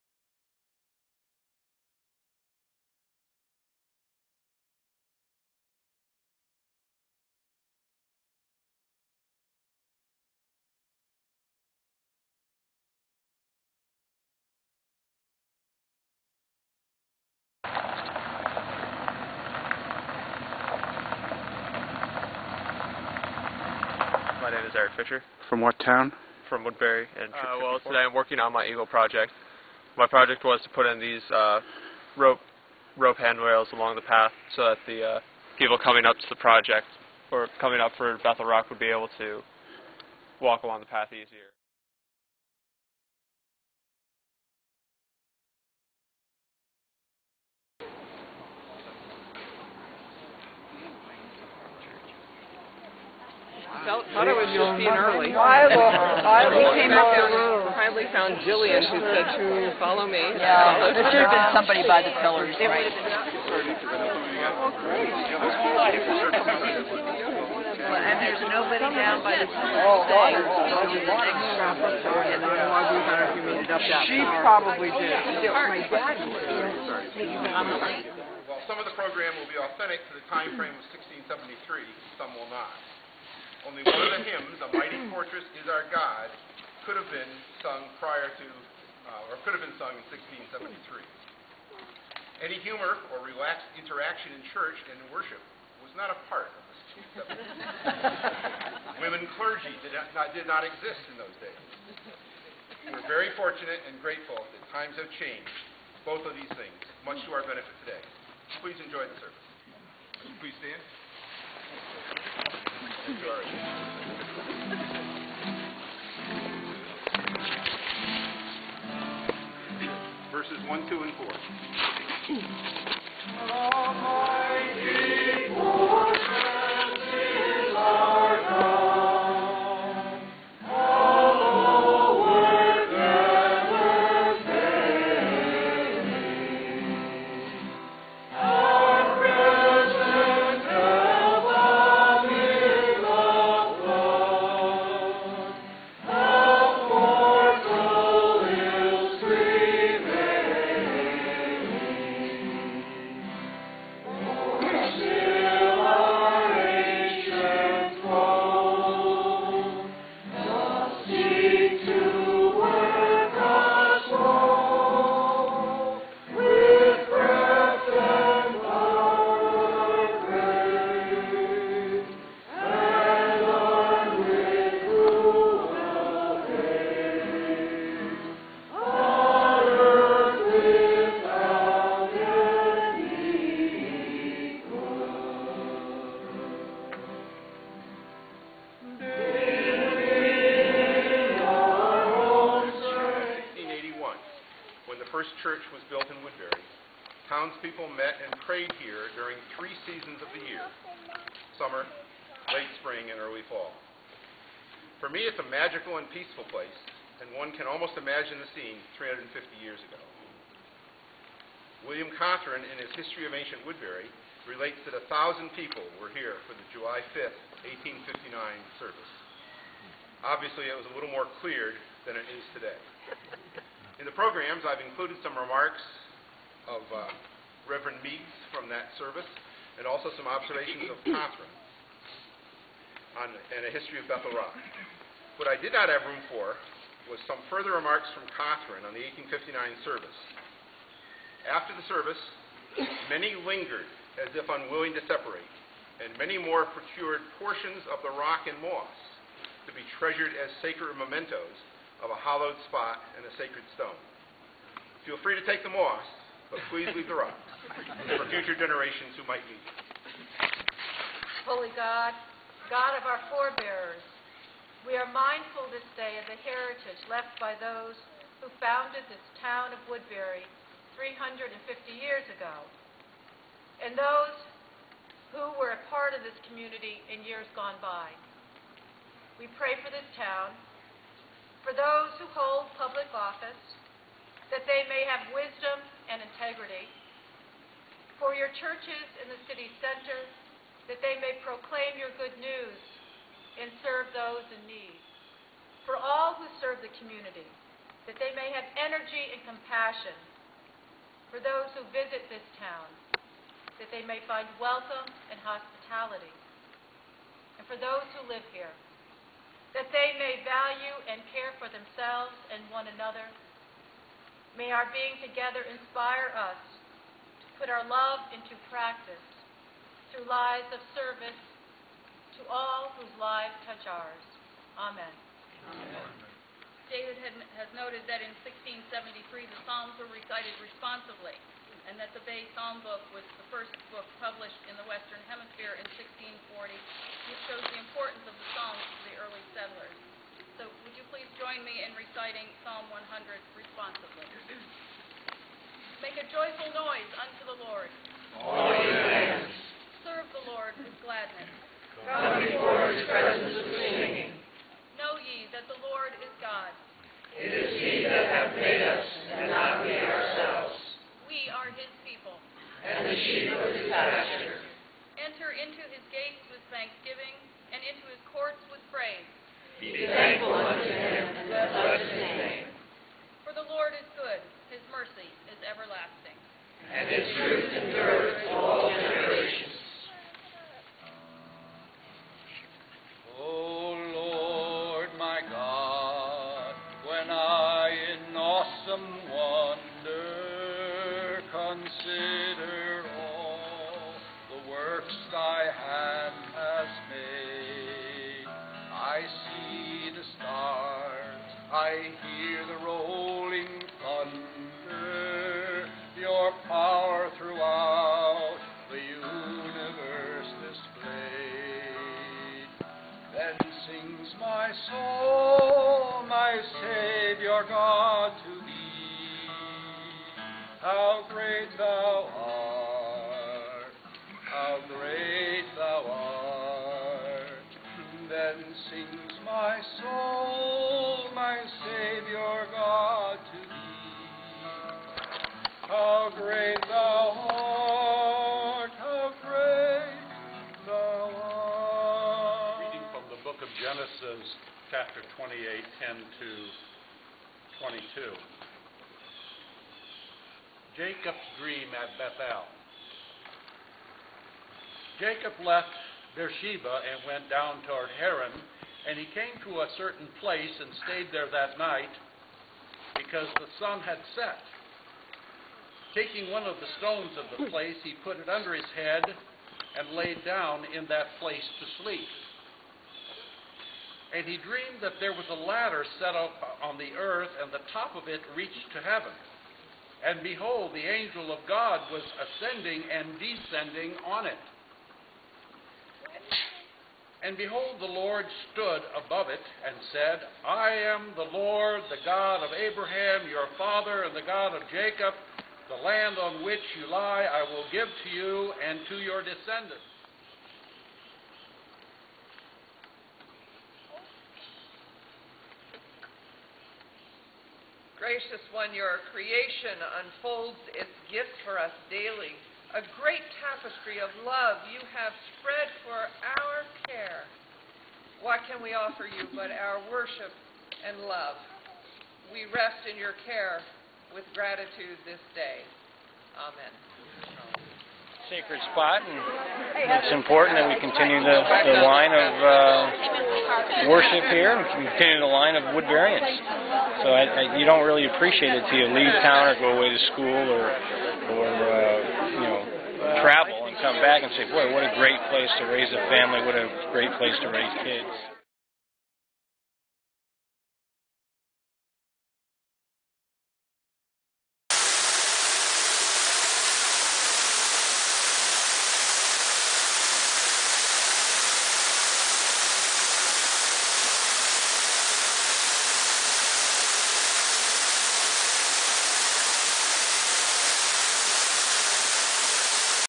**WOODBURY"S 350th**Ecumenical Church Service at Bethel Rock Sept. 20, 2009
On July 5, 1859, 1000 people gathered at Bethel Rock to worship and celebrate the 200th anniversary of the founding of Woodbury, CT. On Sunday, September 20, 2009, fewer that 100 people gathered at the same spot, but peoples' emotions were probably not lessened by the smaller congregation.

This video is edited from the 45-minute outdoor church service. A full-length video of this service, as well as extended video of the entire 350th celebration will be donated to install in the Time Capsule before it's buried at the end of the month. Copies will be available at Video World and the Library also.